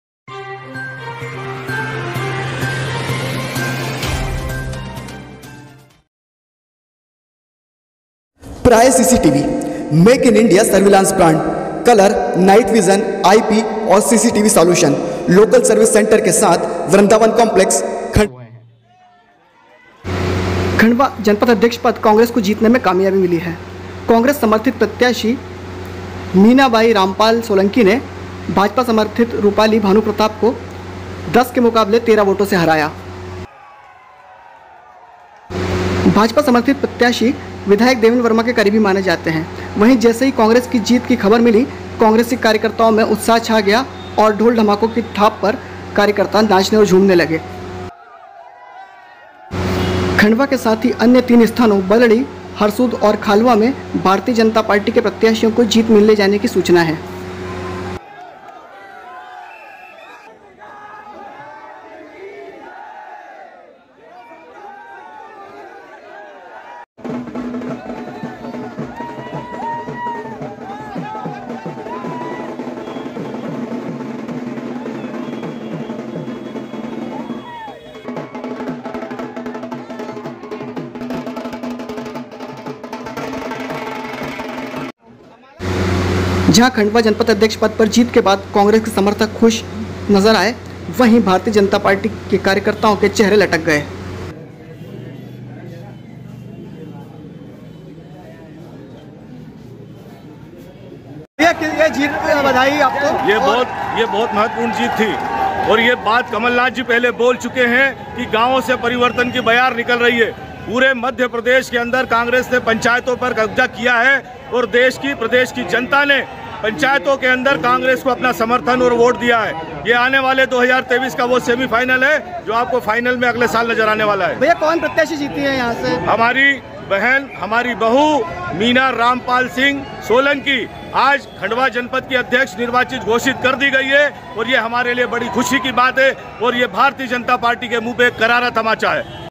Khandwa- जनपद अध्यक्ष पद पर कांग्रेस समर्थित मीना रामपाल जीती,हरसूद खालवा बीजेपी के खाते में..
खंडवा जनपद अध्यक्ष पद पर कांग्रेस समर्थित मीना रामपाल जीती,हरसूद खालवा और किल्लौद बीजेपी के खाते में जाने के समाचार।@NewsfirstKnw